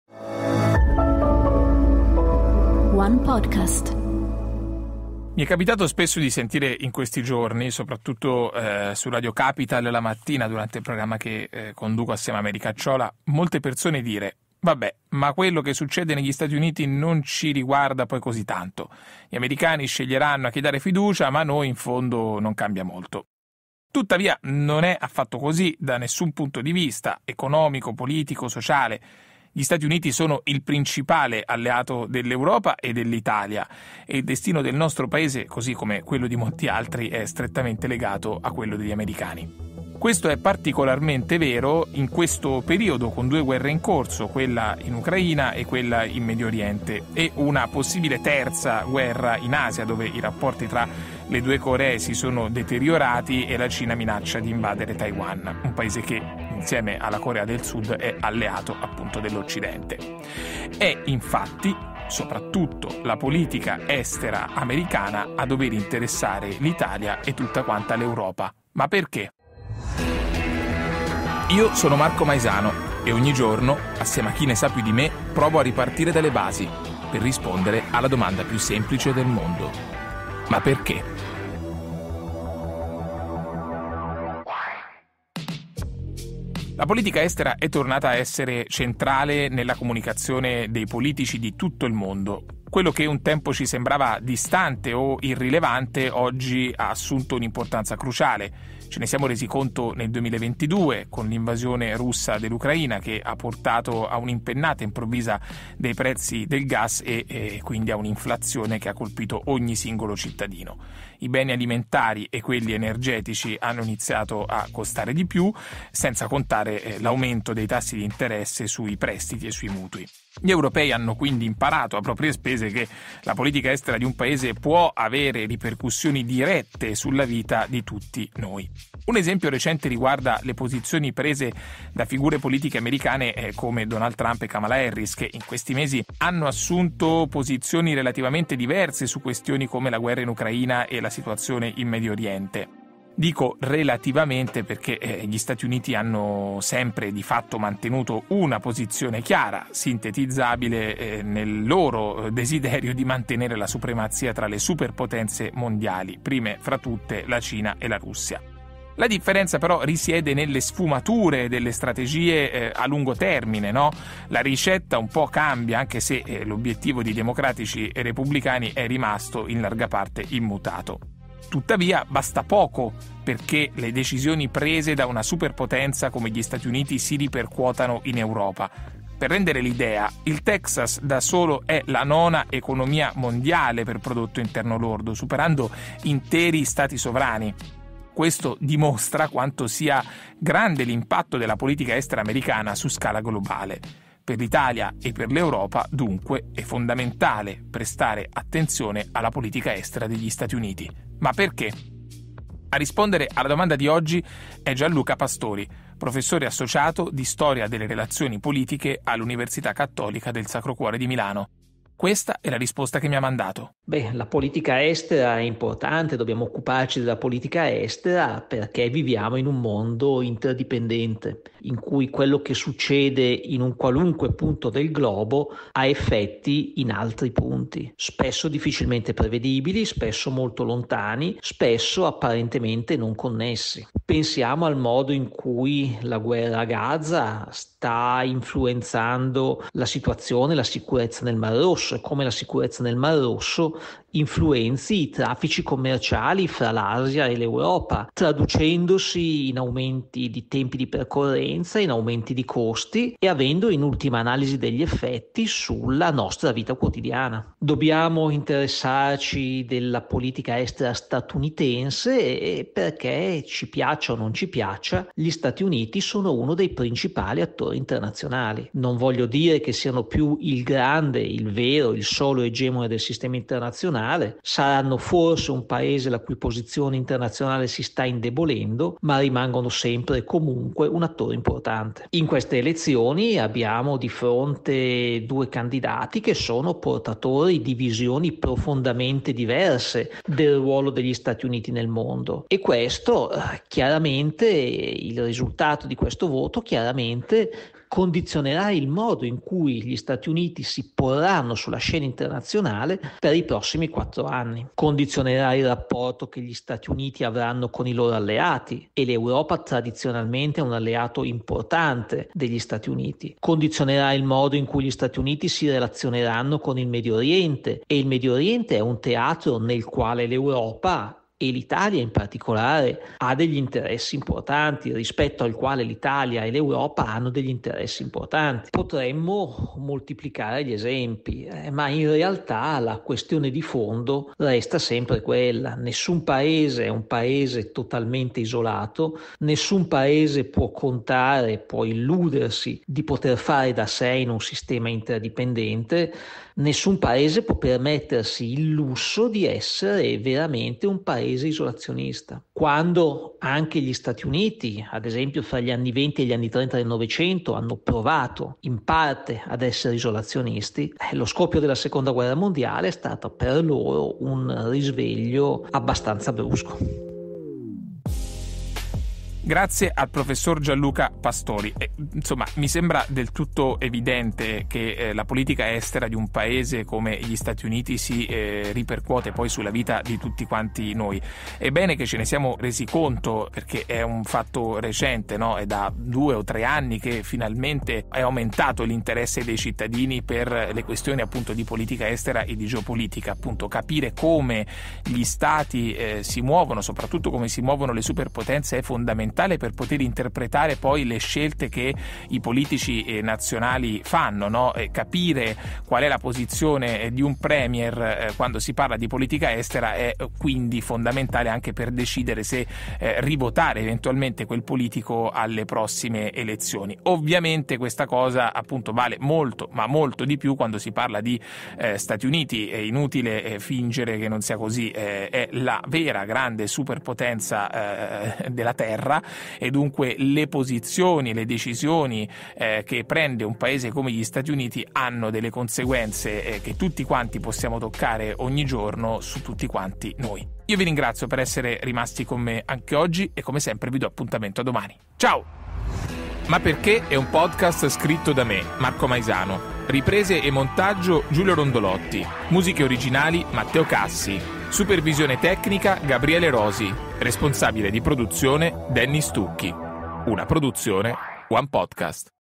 500 | Ma perché deve interessarci la politica estera americana?
Spesso sento dire: “Quello che succede negli Stati Uniti non ci riguarda”. Ma non è così: il destino dell’Italia e dell’Europa è strettamente legato a quello americano, specialmente ora, con guerre in Ucraina, Medio Oriente e tensioni in Asia. La politica estera americana ci coinvolge. Ma perché? Ne parlo con Gianluca Pastori.